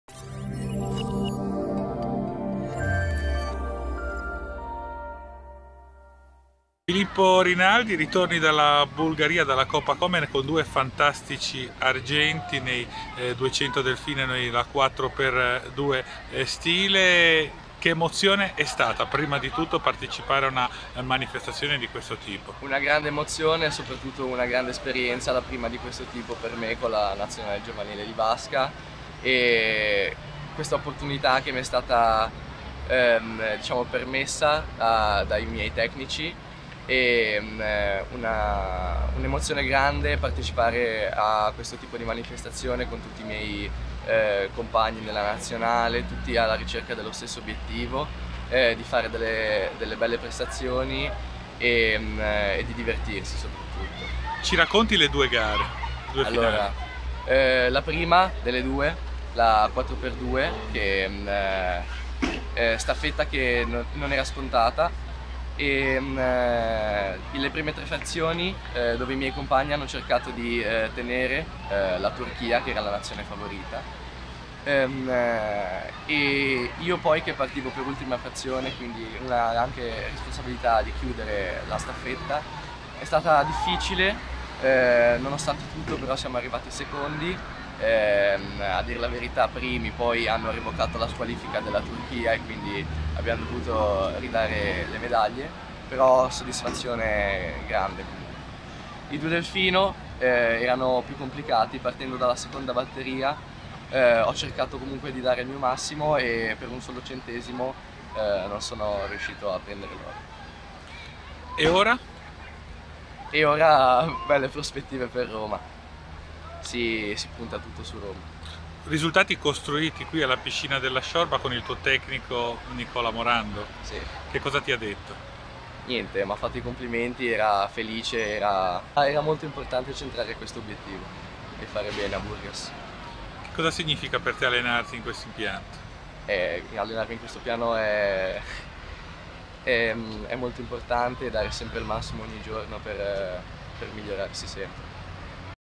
Filippo Rinaldi due volte d'argento alla Coppa Comen
Ottima risposta di Filippo Rinaldi alla sua prima convocazione in Nazionale giovanile con la quale ha partecipato alla Coppa Co. Me. N svoltasi a Burgas in Bulgaria dal 21 al 23 Giugno.